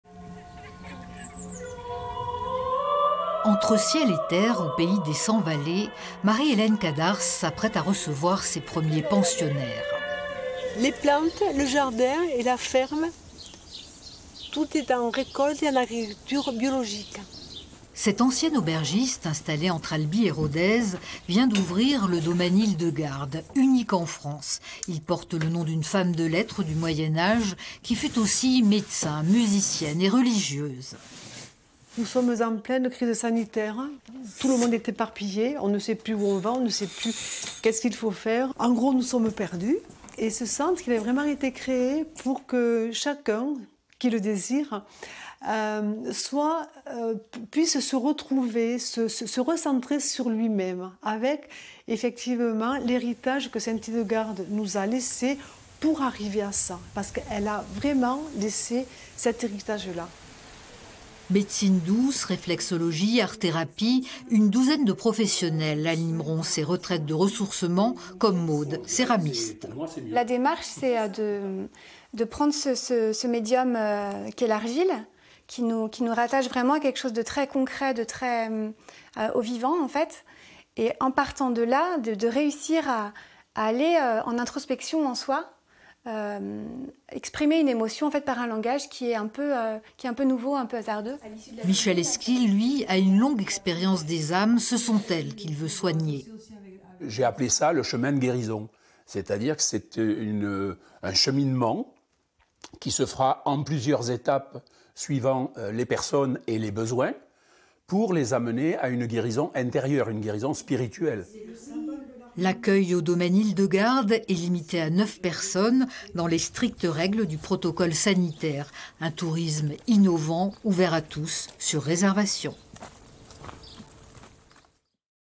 Aveyron : entre Albi et Rodez, le Domaine Hildegarde s'apprête à ouvrir ses portes.
Ce centre de ressourcement, unique en France, est fondé sur les principes de Sainte-Hildegarde de Bingen, femme-médecin du Moyen-Age. Installé à La Vialette sur la commune de Camjac, il va accueillir ses premiers pensionnaires pour des retraites spirituelles animées par une douzaine de professionnels.